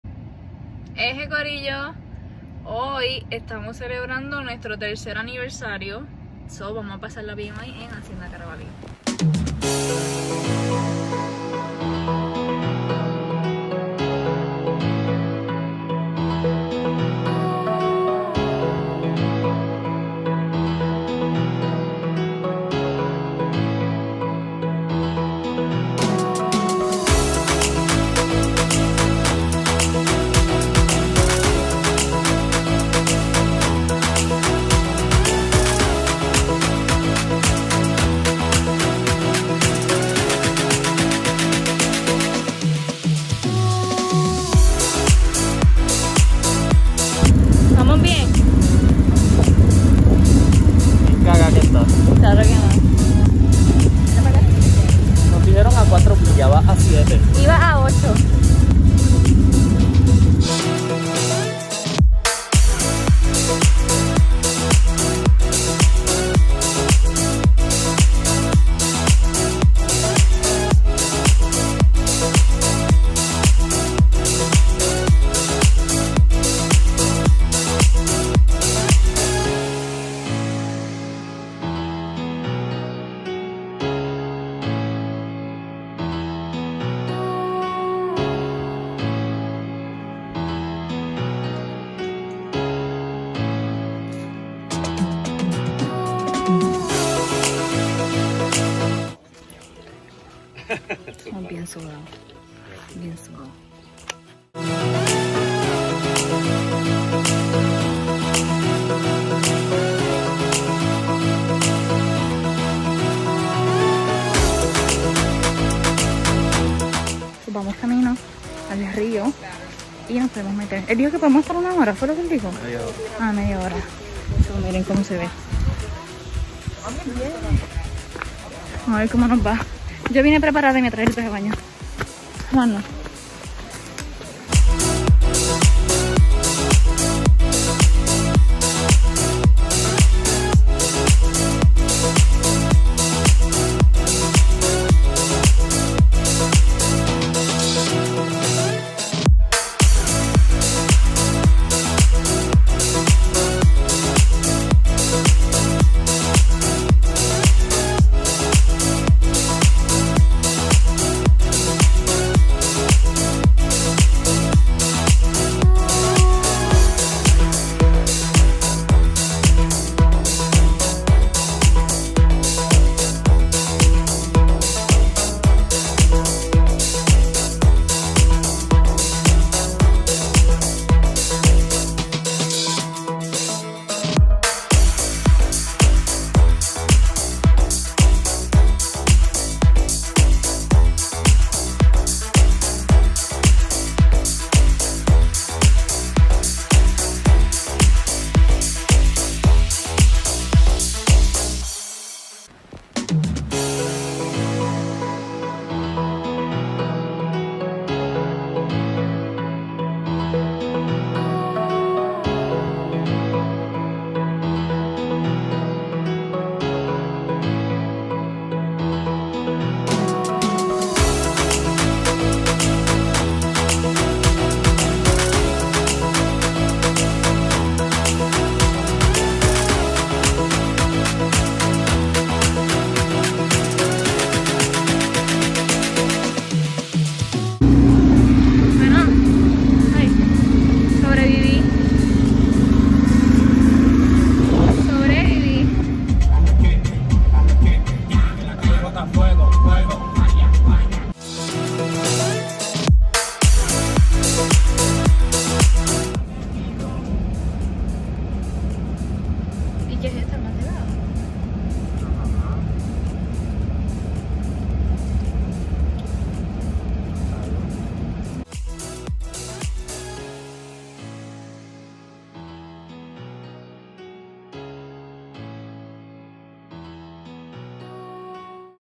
Vlog #2: Aniversario en Hacienda Carabali
Celebramos nuestro tercer aniversario en Hacienda Carabali!